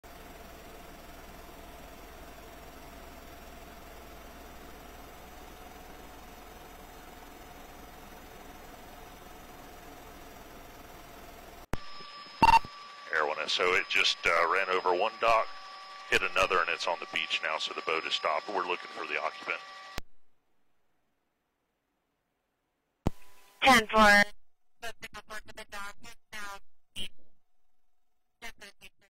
Driverless boat in Florida spins out of control
Three people conducting a photo shoot on a boat in a busy Florida river fell overboard Saturday, leaving the vessel unmanned and out of control. (Oct. 12)